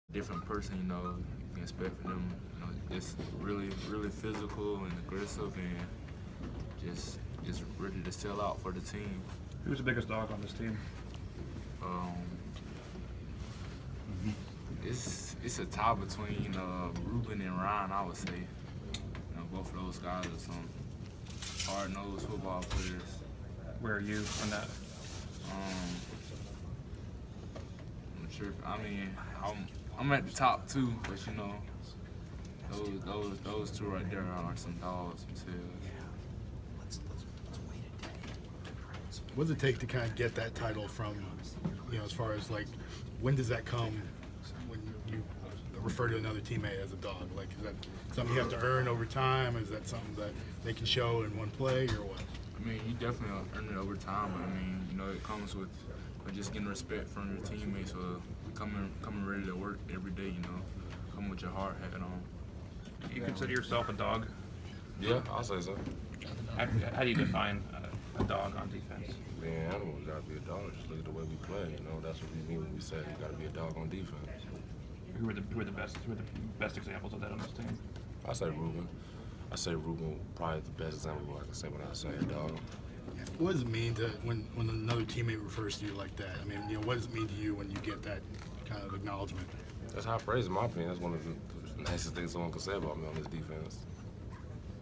What it means to be a dog on Alabama defense
Alabama football players have a name for the nasty players on defense. Shaun Dion Hamilton and Jonathan Allen explain.